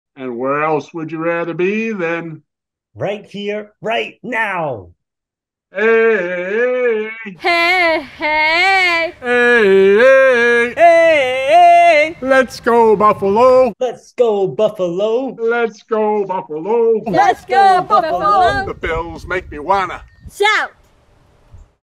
Get ready for the season!
Join our Bills Speak group in singing our theme song!